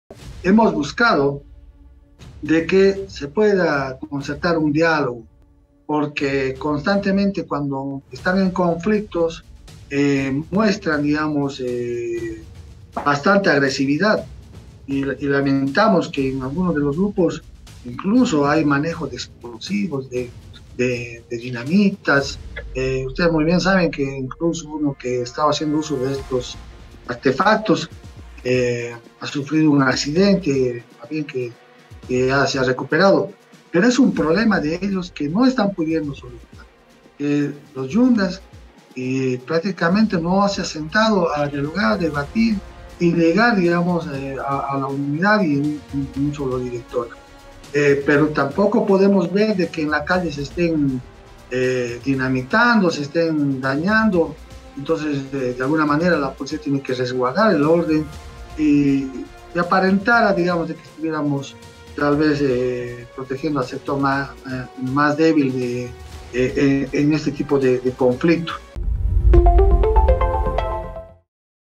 Diálogo para solucionar conflictos en LaPaz
El ministro de Desarrollo Rural y Tierras, Remmy Gonzales Atila, prioriza el diálogo para solucionar los conflictos en Adepcoca.
Vía: Bolivia tv